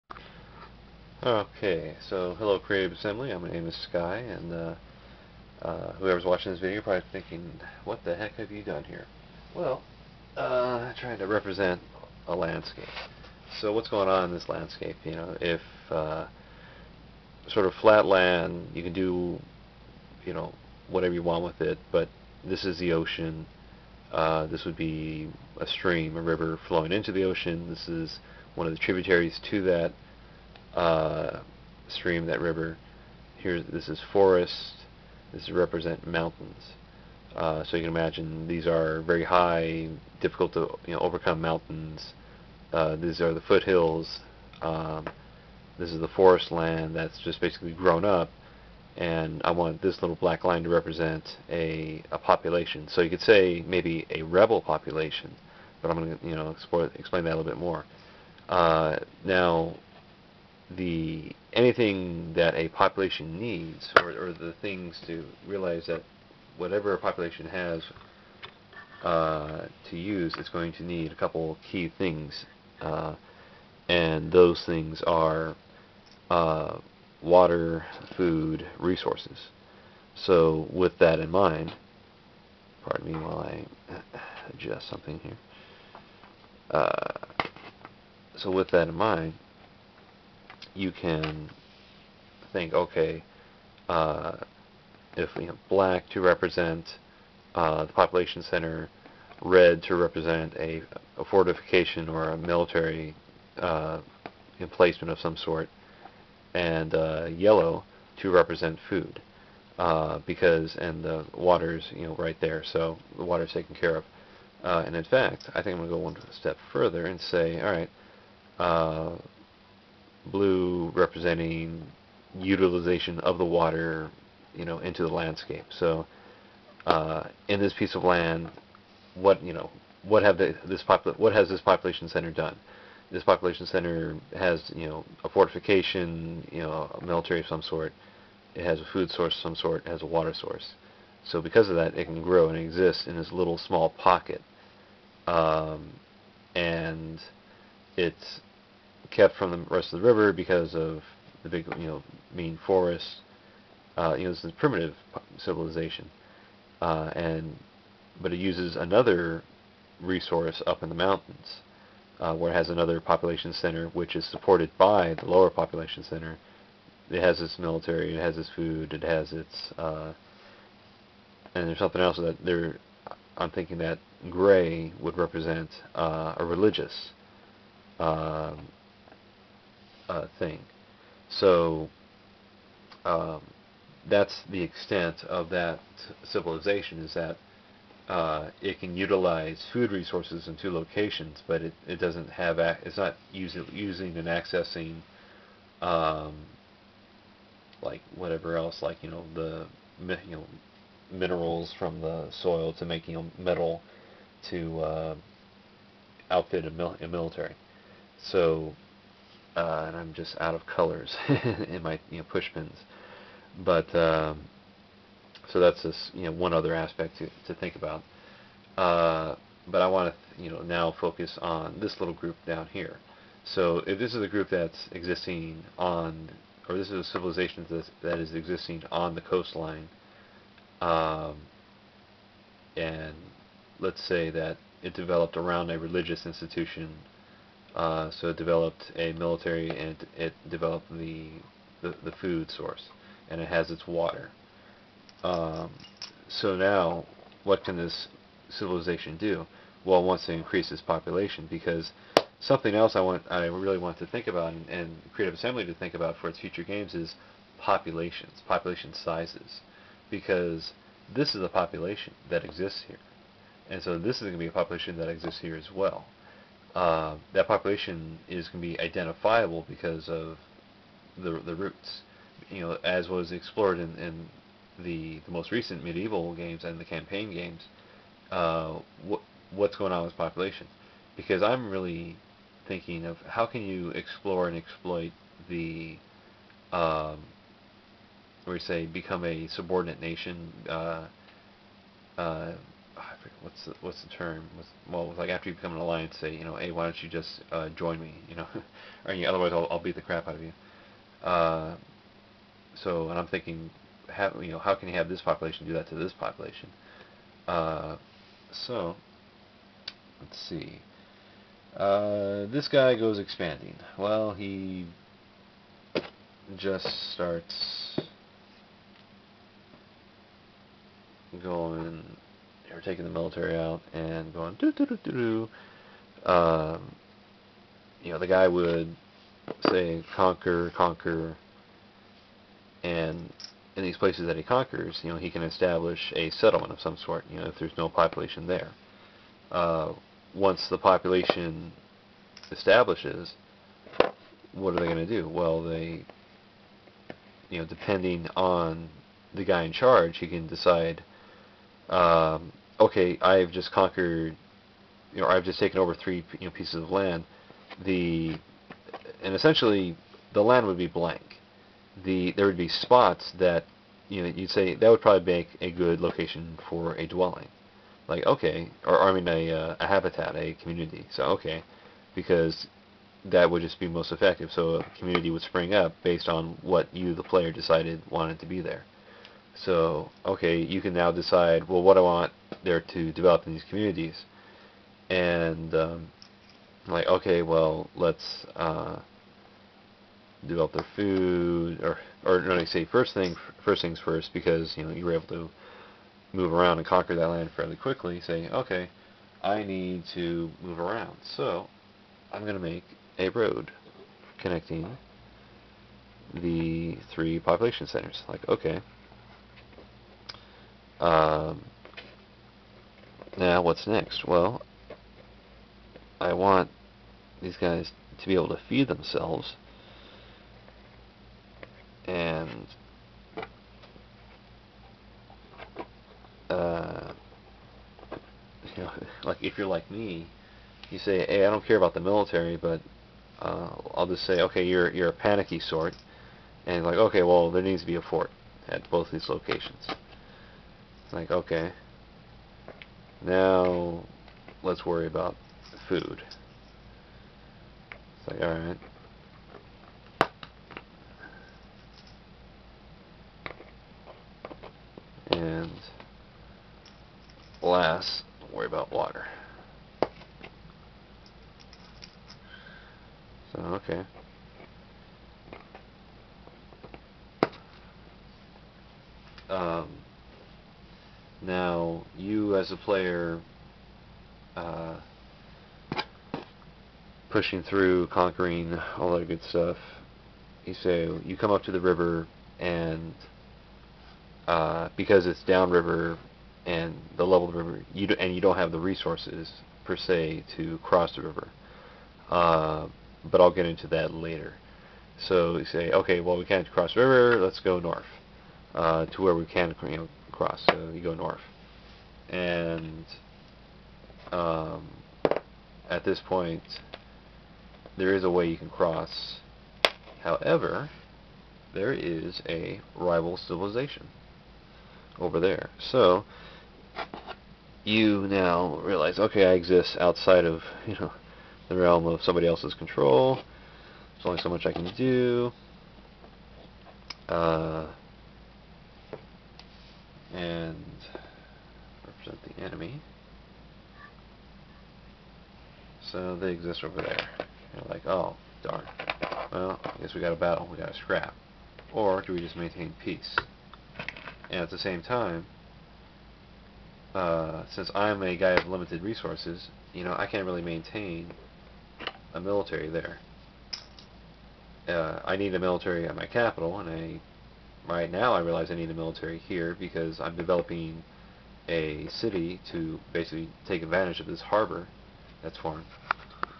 A Message to Creative Assembly: Ideas for Rome 2. Pt 2
Layout of the land and concepts of new landscape features.

Each square represents a "county."